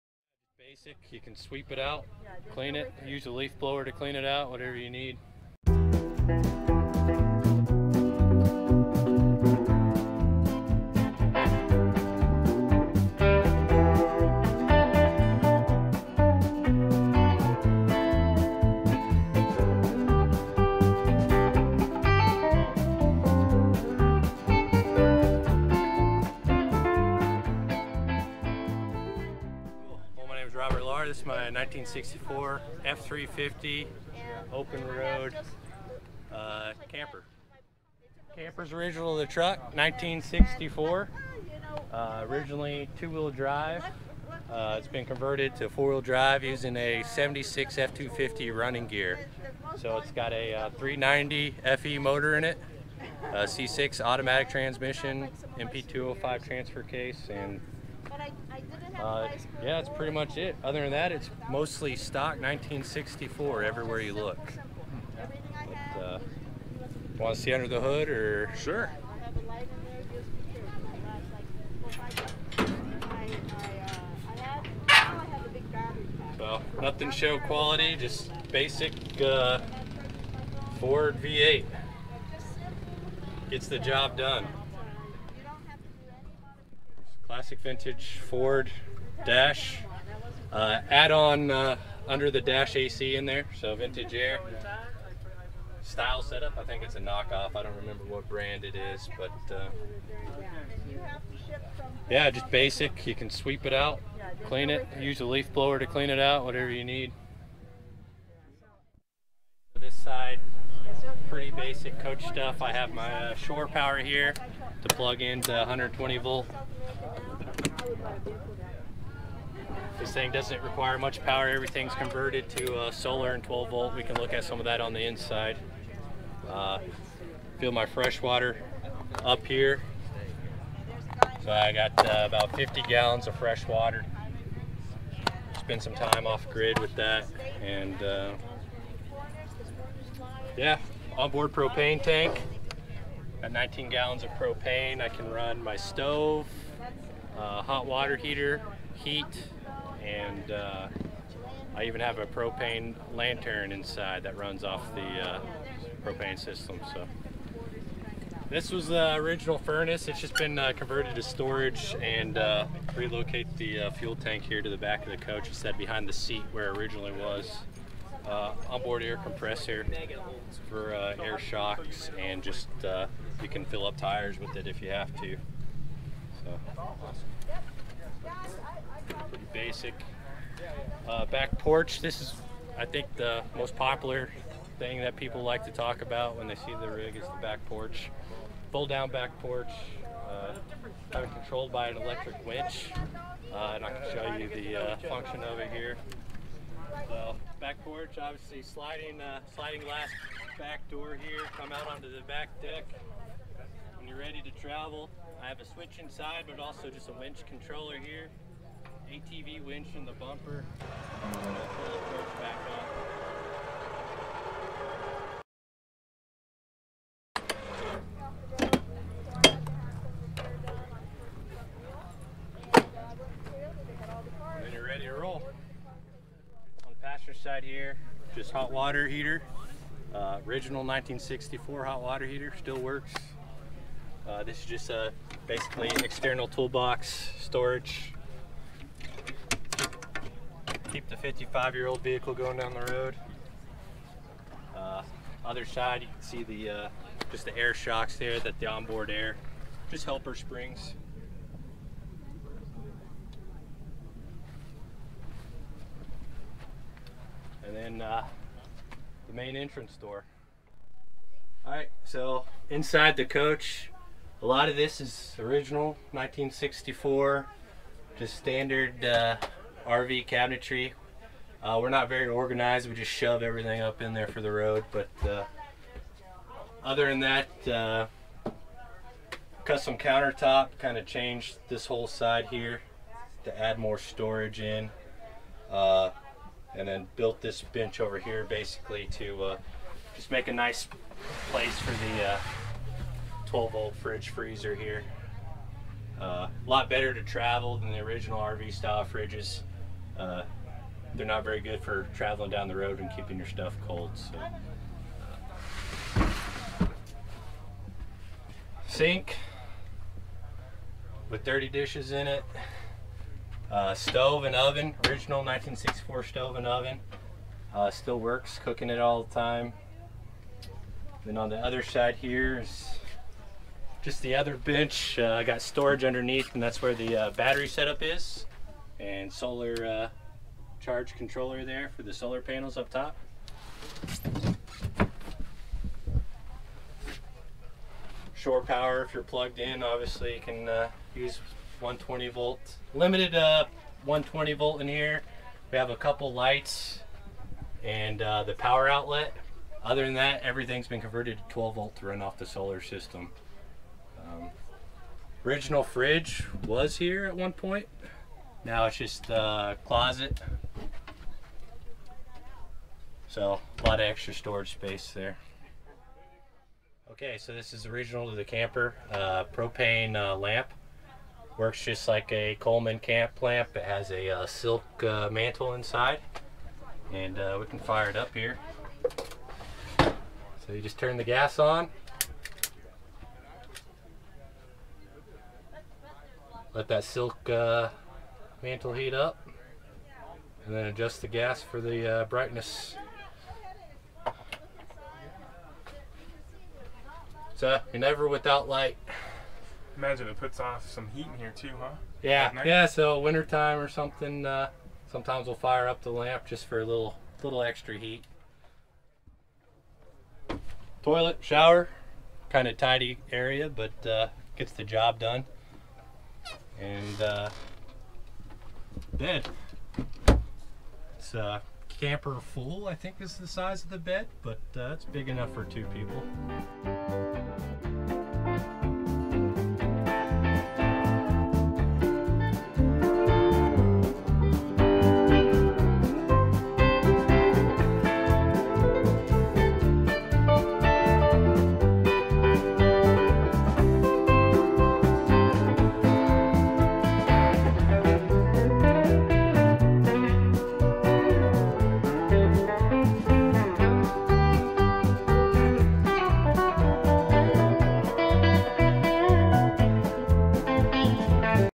Overland Upgraded 1964 Ford F-350 Open Road 4x4 Vintage Motorhome
Time warp back to 1964 in Robert Lahr's Ford F-350 Open Road Motorhome which was originally built straight from the factory.  Being at Overland Expo East 2019, it's of course had some overland upgrades, including the addition of 4wd, some light trail armor, solar power and more. But the aesthetics masterfully retain all the good traits that 1964 design had to offer.  The interior is just as impressive as the exterior - there's even an original propane lantern inside. I hope you enjoy this walkaround as much as I did!